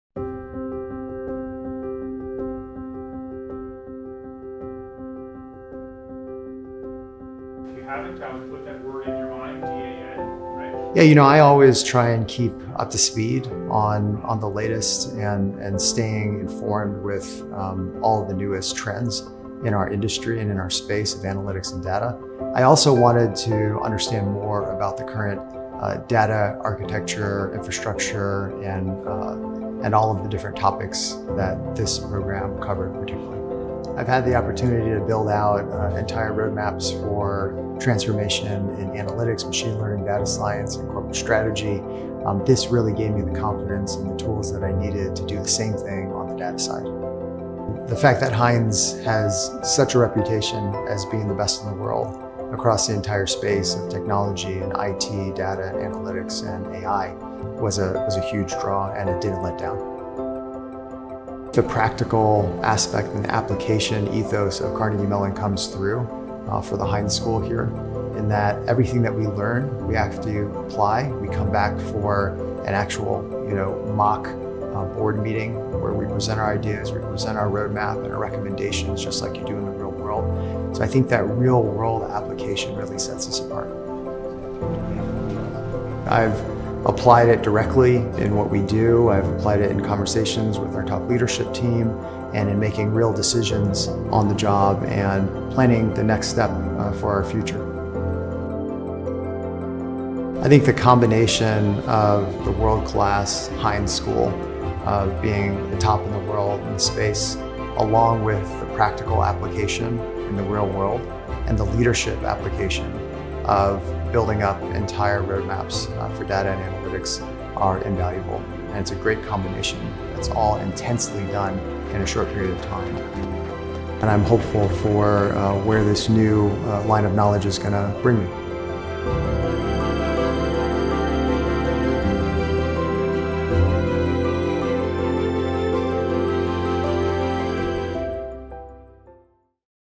CDO Certificate at Carnegie Mellon - Chas Murphy, Disney Entertainment
Carnegie Mellon University's reputation as the best place in the world to study data, IT, analytics and AI "was a huge draw," Chas Murphy said. "And it didn't let down."

Murphy, the Senior Vice President of Data and Analytics for Disney Entertainment's streaming services, earned his Chief Data Officer Certificate from Carnegie Mellon's Heinz College. The program gave him the confidence to build roadmaps for corporate analytics strategy and kept him current on the latest industry trends in data architecture and infrastructure.

CMU's Chief Data Officer Certificate Program (CDataO) provides current and future CDOs with the critical strategic insight, management techniques and analytical capabilities you need to set up a 21st-century data governance program.